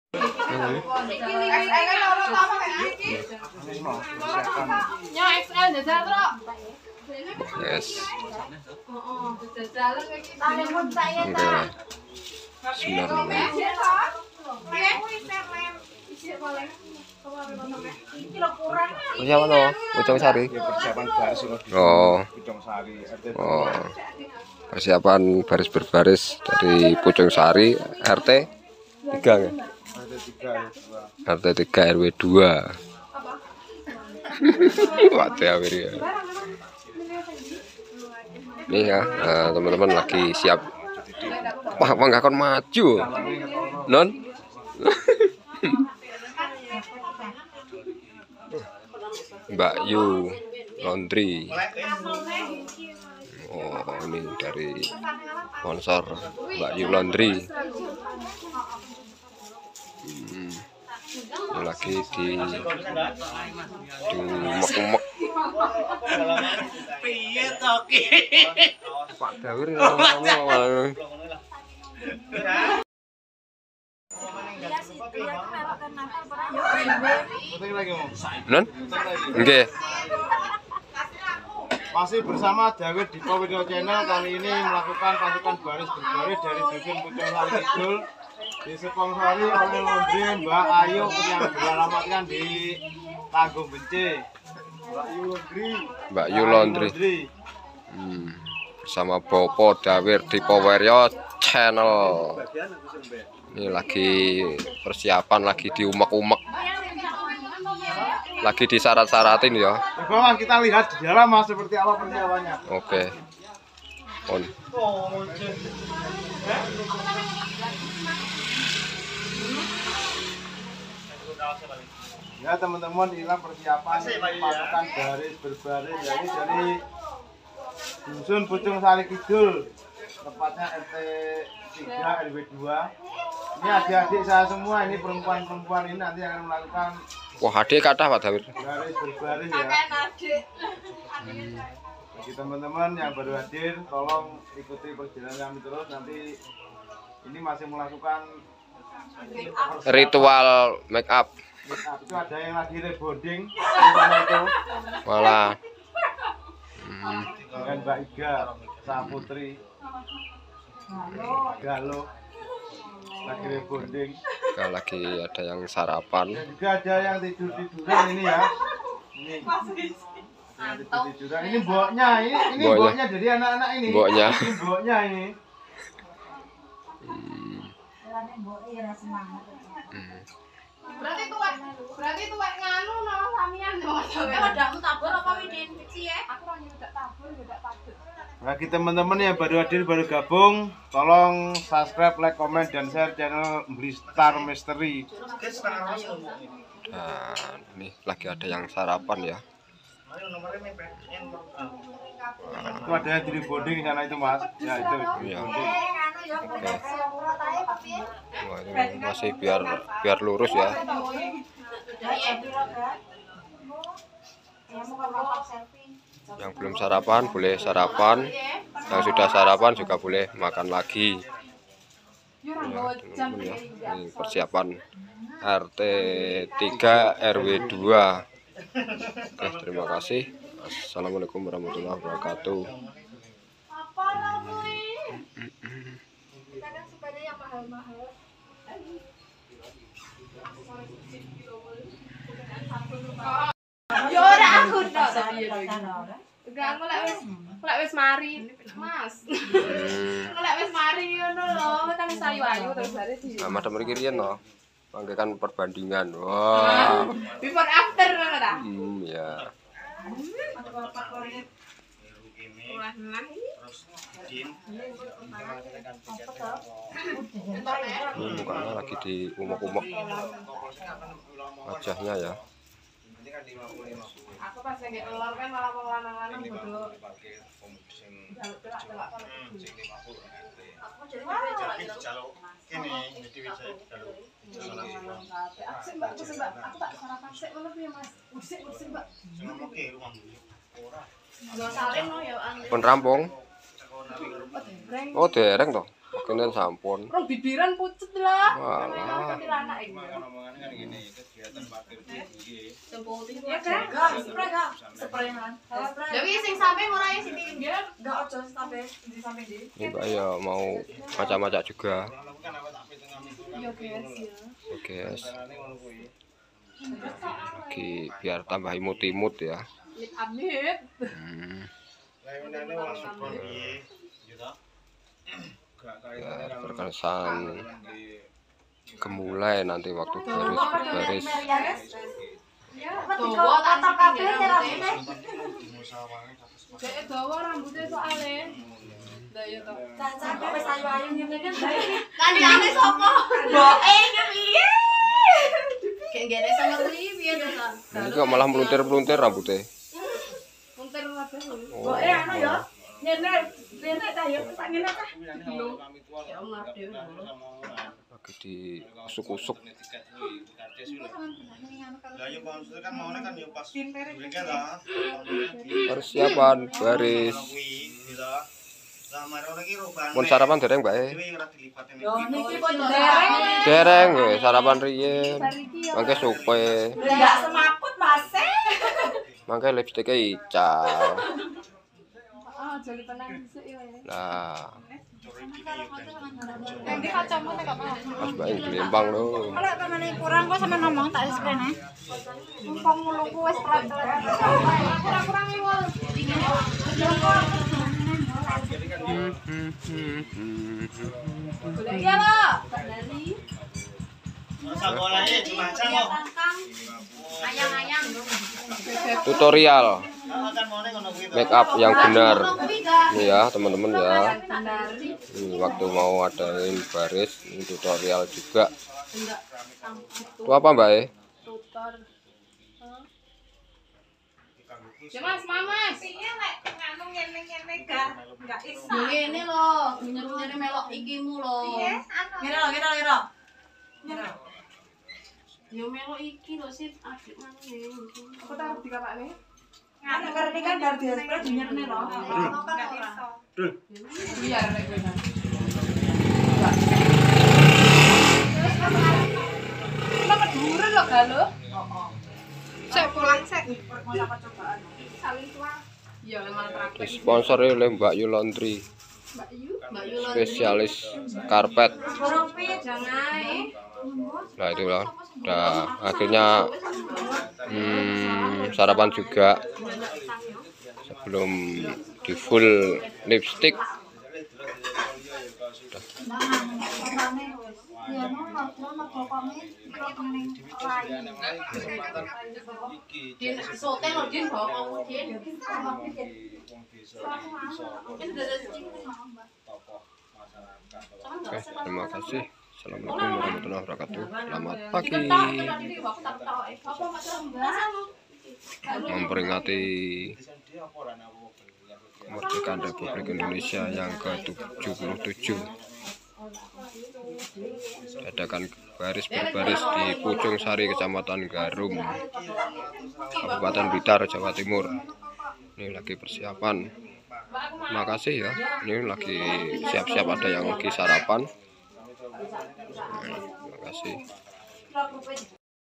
lomba baris berbaris dan persiapan dalam merias diri
Persiapan baris berbaris dalam rangka memperingati HUT RI KE 77 th 2022 di pucungsari slorok garum blitar, ikut memeriahkan hari kemerdekaan republik indonesia, NKRI HARGA MATI